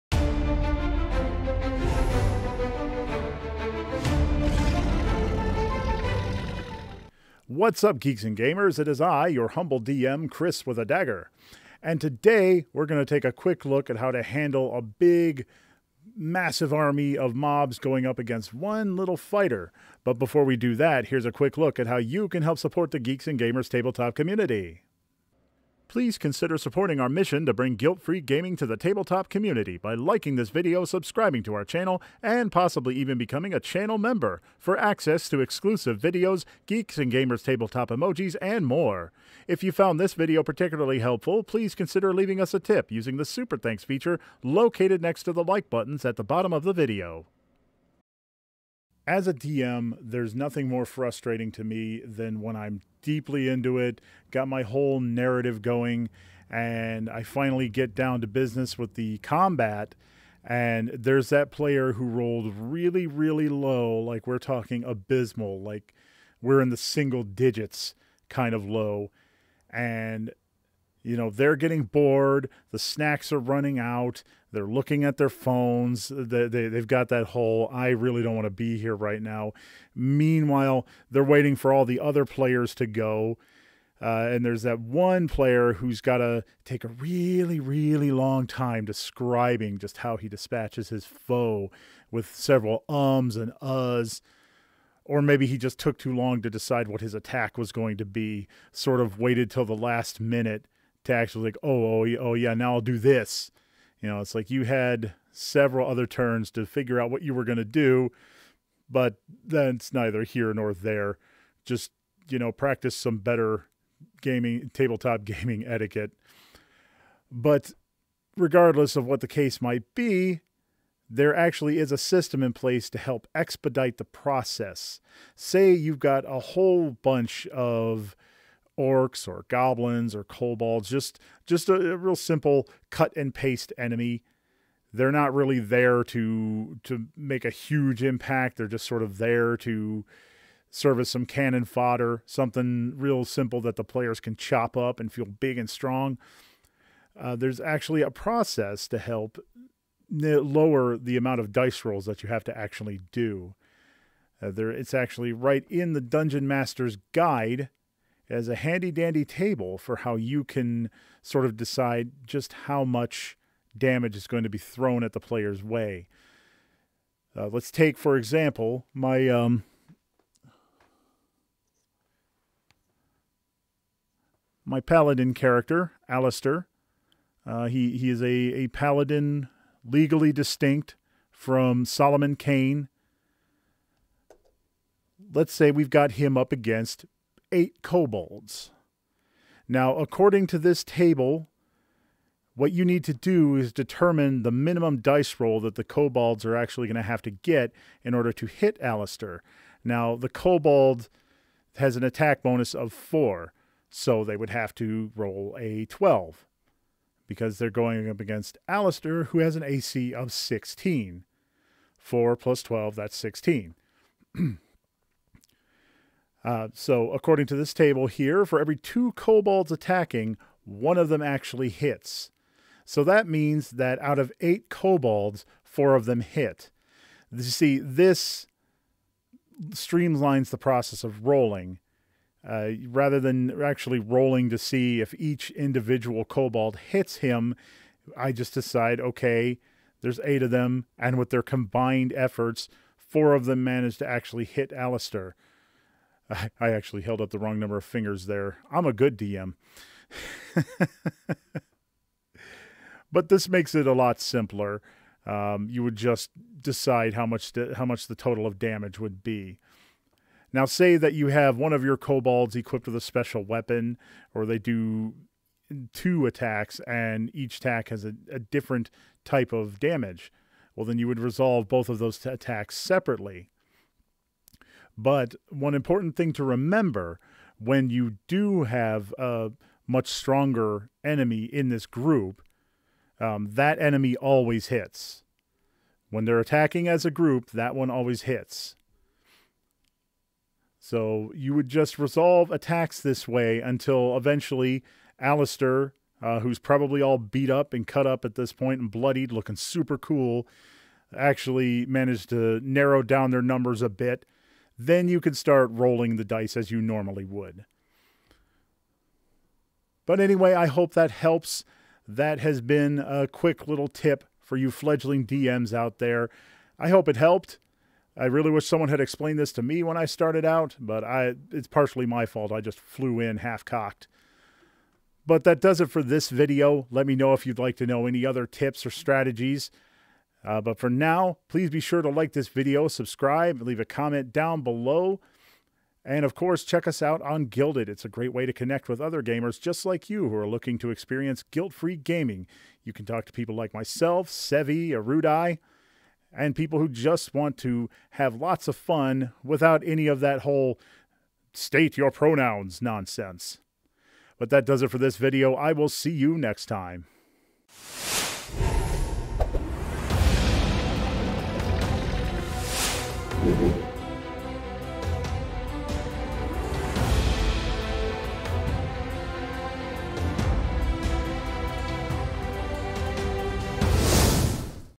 D&D 5E: Handling Mobs
Our D&D 5e content creator, Kris with a Dagger, discusses how to efficiently run mobs in 5E! Check out this video to learn more.